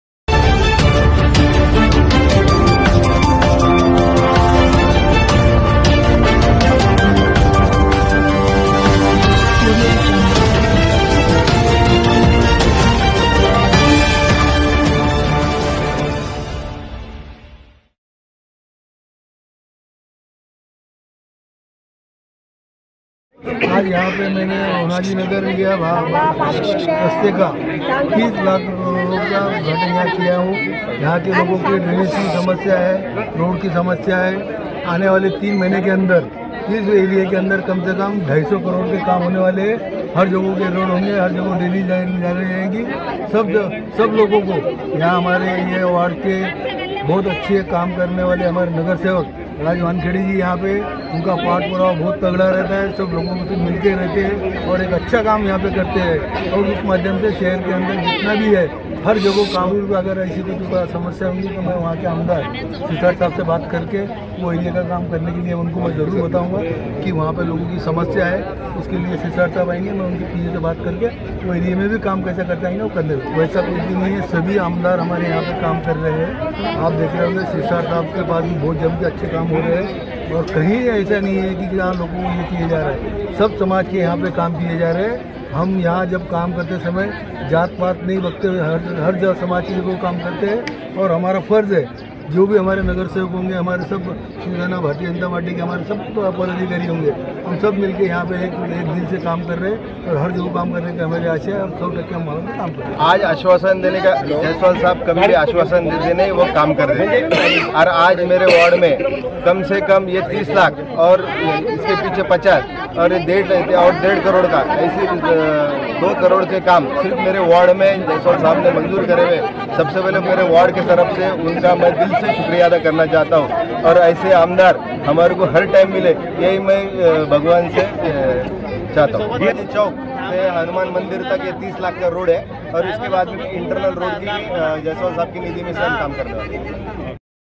औरंगाबाद मध्य विधायक प्रदीप जैस्वाल द्वारा विकास कामों का सिलसिला जारी
*A4u tv channel from sham e rahbar urdu daily*